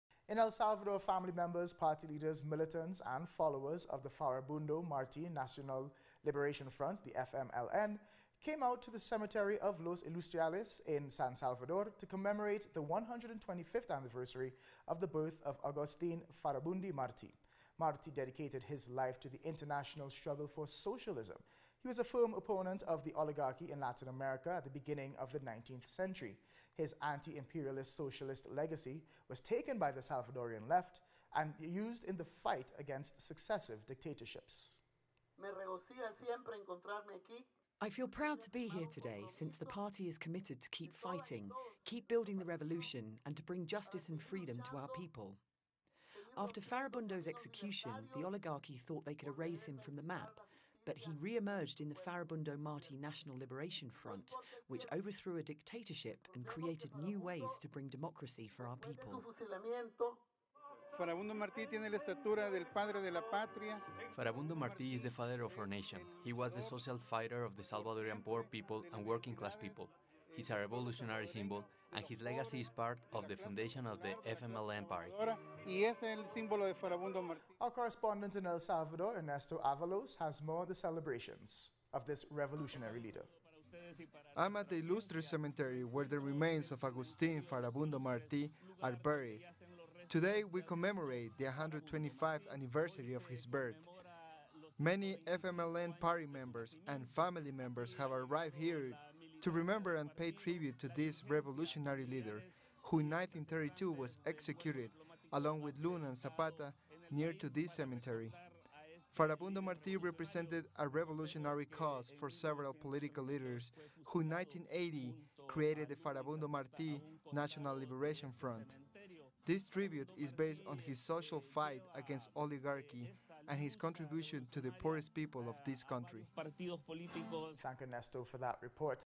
FMLN leader remembered
In El Salvador followers of the Fara-Bundo Marti National Liberation Front (FMLN),  commemmorated the 125th anniversary of the birth of Agustín Fara-Bundo Martí.